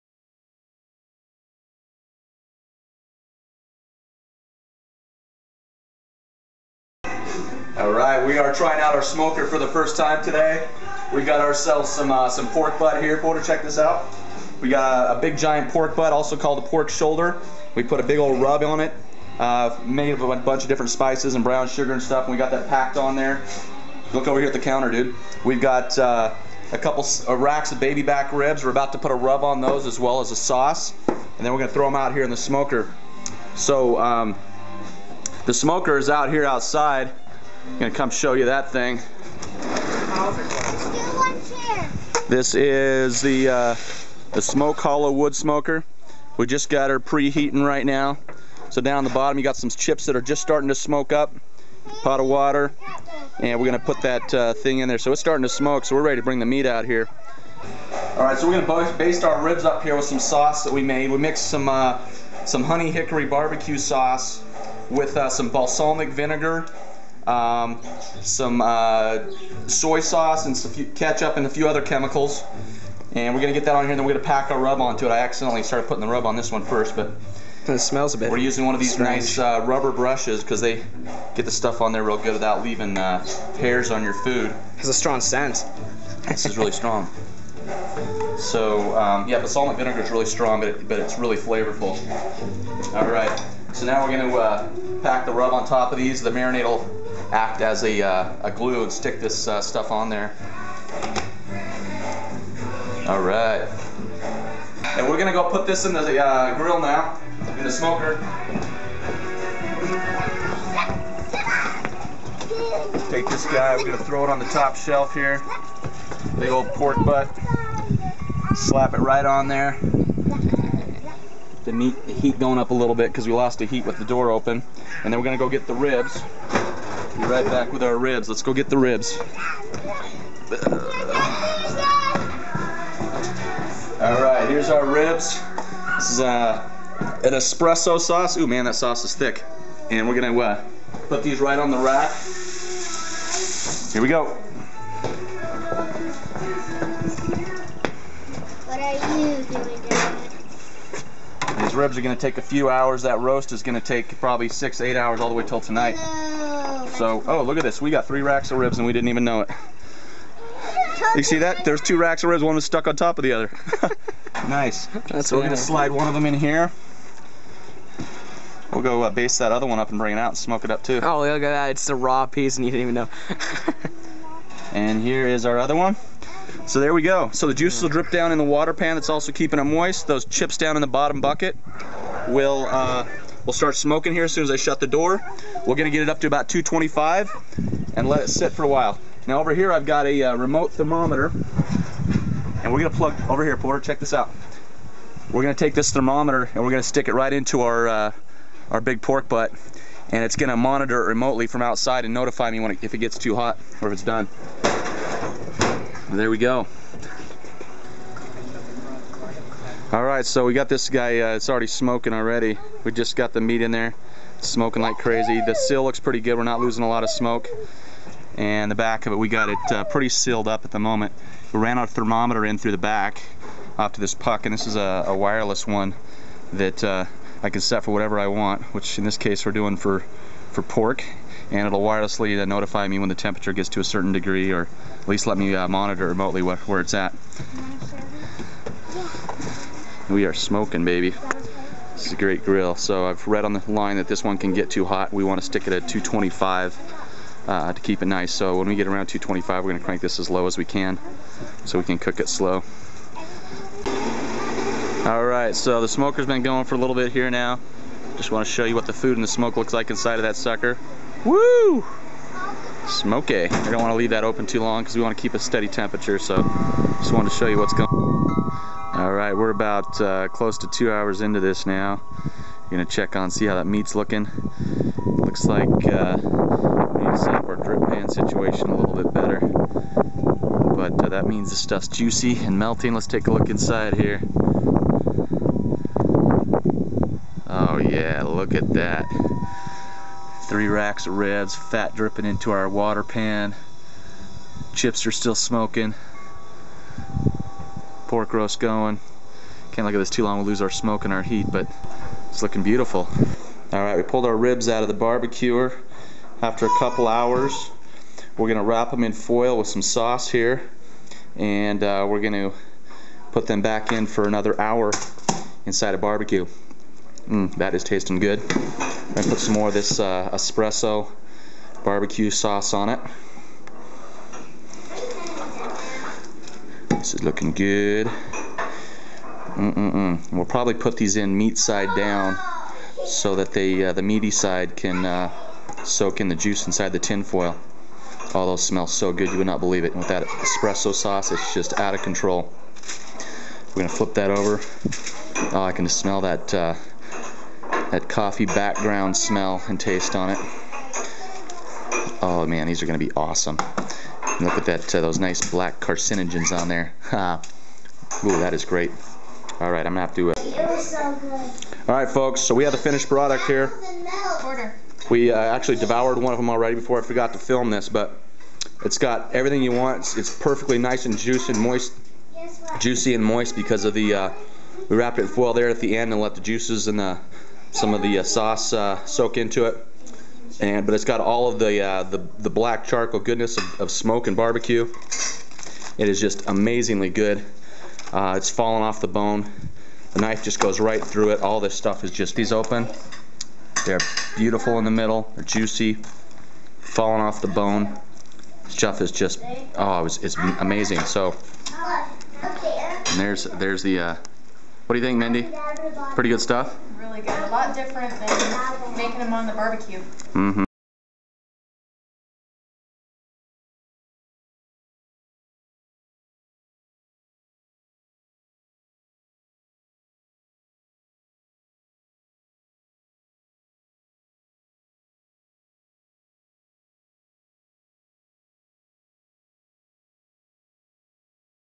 First Time Smoking Meat
Smoking Pork Ribs with "Smoke Hollow" by Outdoor Leisure